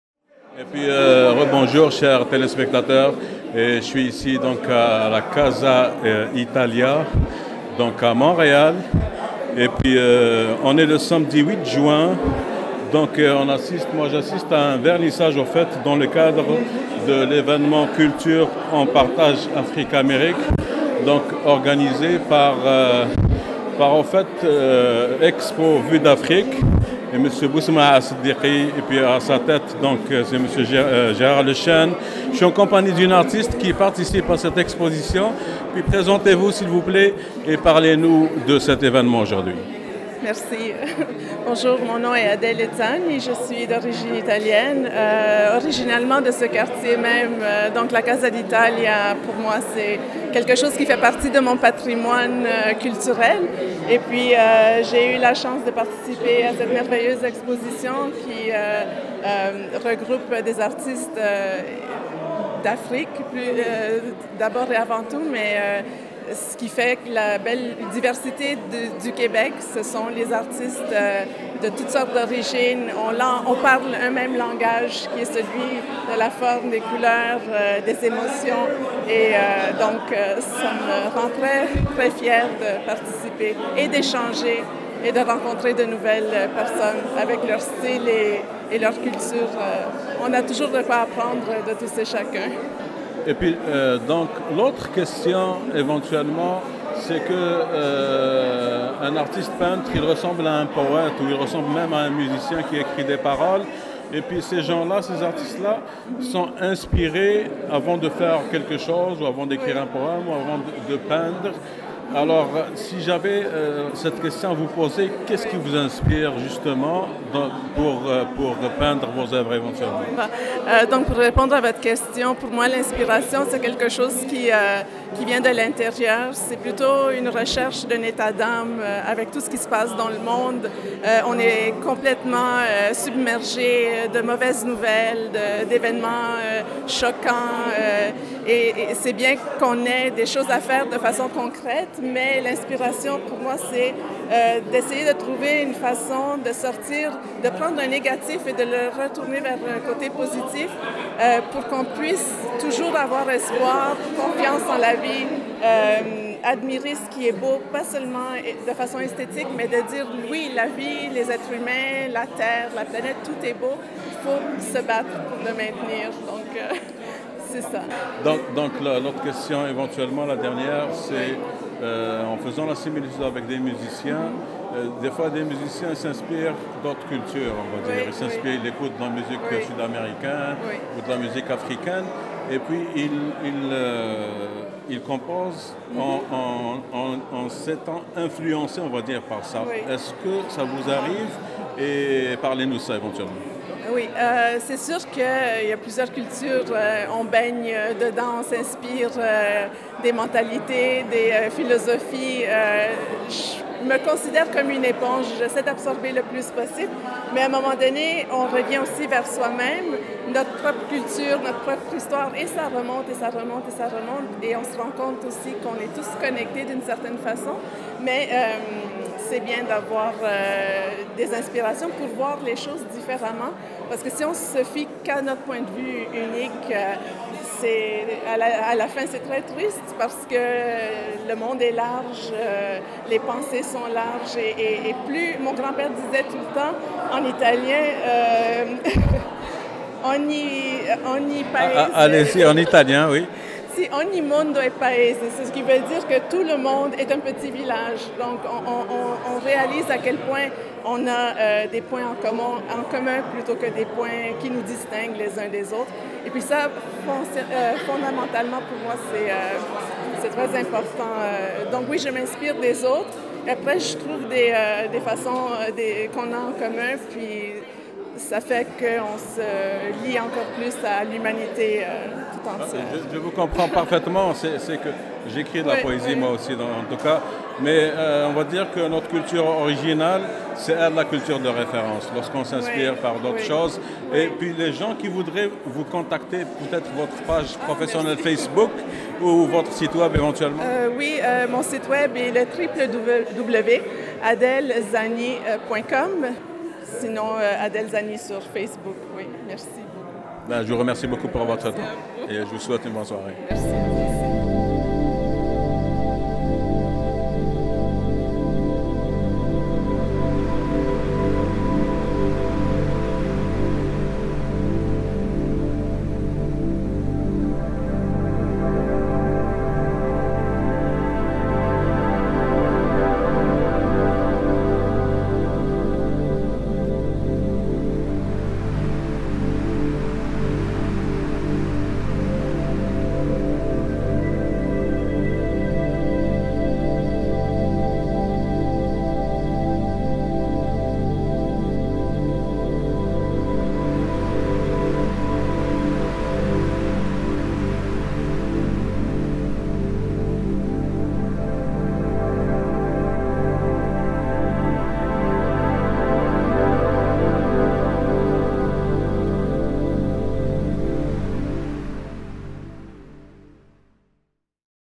Le vidéo journaliste Akim Kermiche interview l'artiste peintre Adele Zanni
Lors de l'exposition collective CULTURE EN PARTAGE AFRIQUE - AMÉRIQUE
Présentée par NOVIM ARTS ET MATIÈRE
À la Casa Italia - Montréal - Québec - Canada - Le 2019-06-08